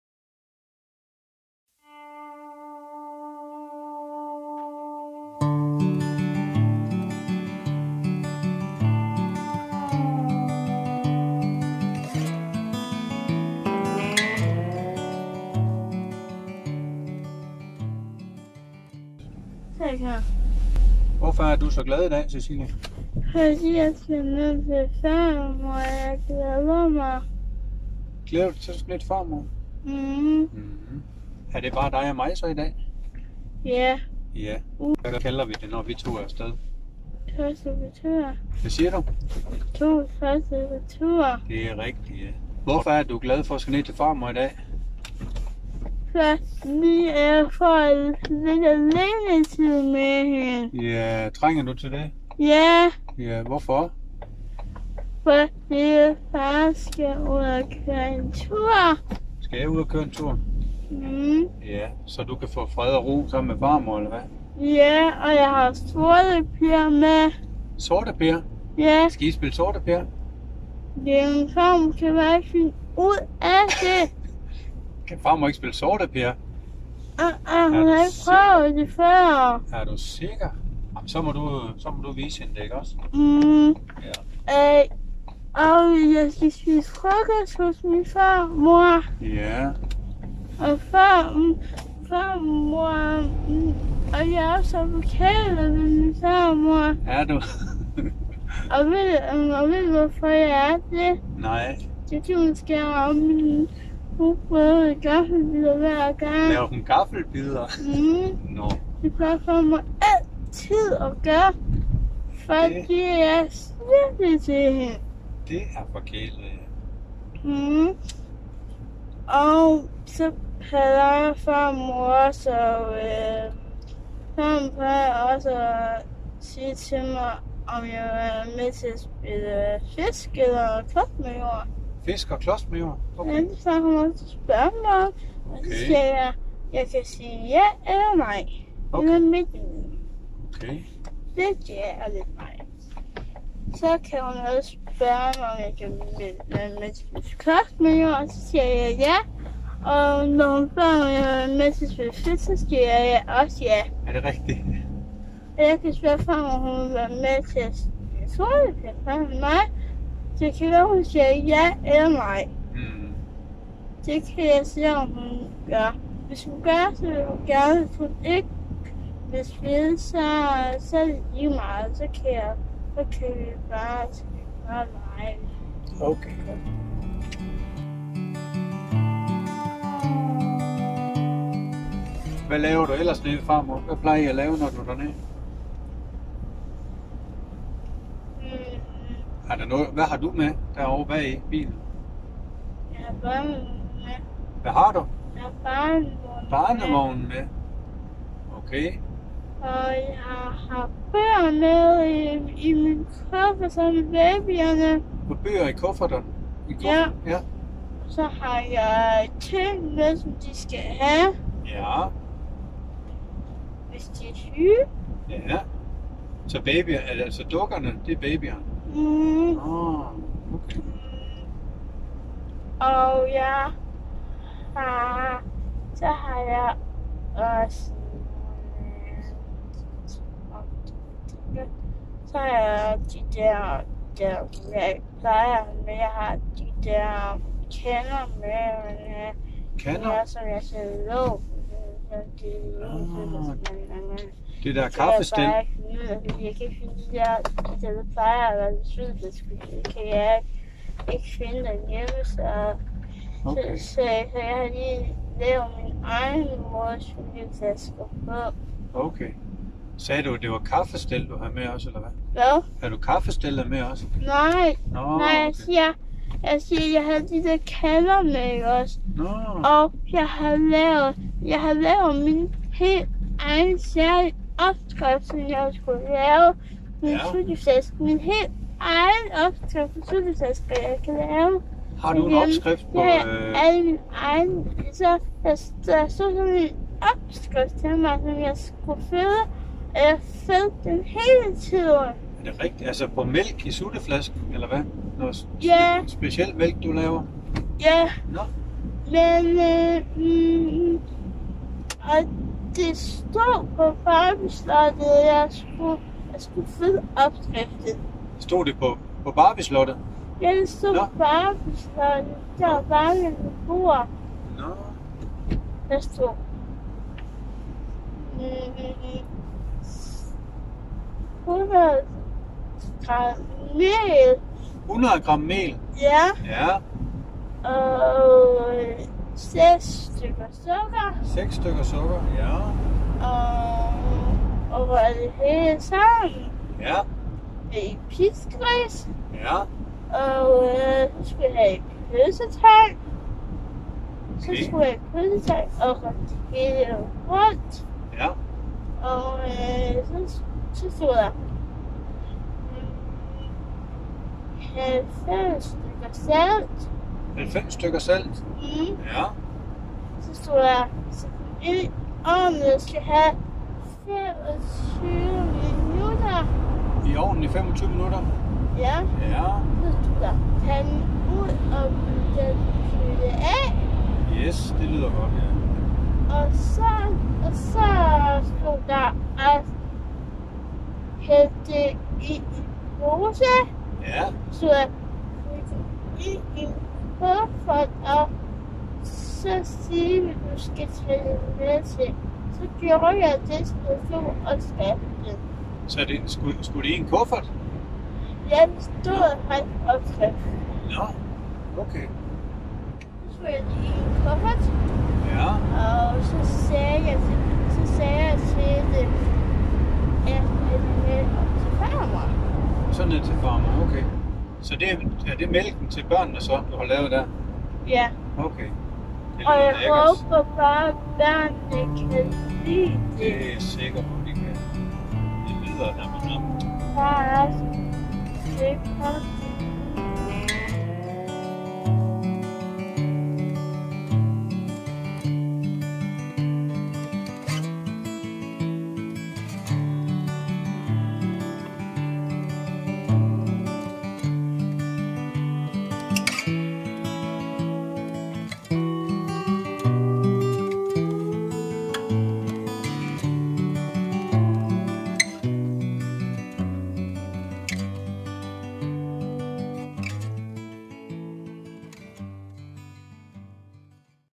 Cecilie Oplyser Verden_Afsnit 2_Cecilie og Farmor
Cecilie har er født med Myotoni (middelsvær muskelsvind). Hun er en særlig pige med en stor fantasi samt - ikke mindst - en meget veludviklet følelses-intelligens og sansestyrke. I denne serie fortæller hun lidt fra sin hverdag - og oplyser verden om sine helt særlige oplevelser, tanker og følelser.